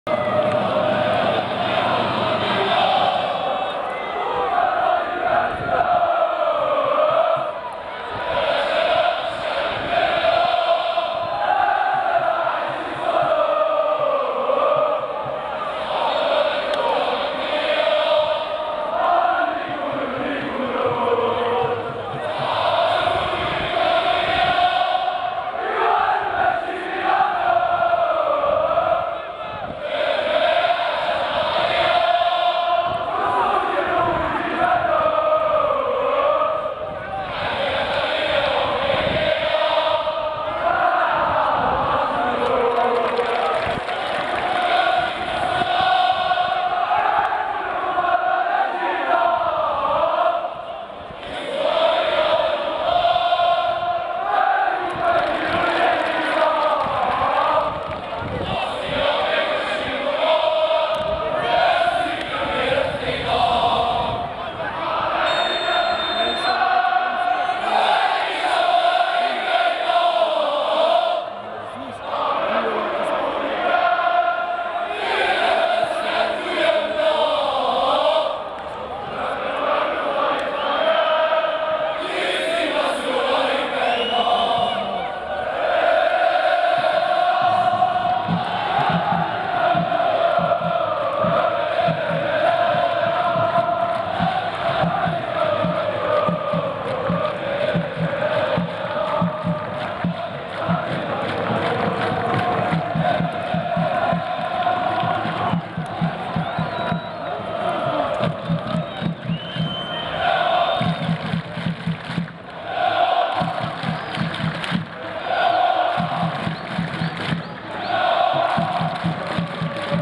Ambiance hebal Hameraoua au stade zabana 2020 vs m'lila
الحمرواة يبدعون في إيشونج بملعب أحمد زبانة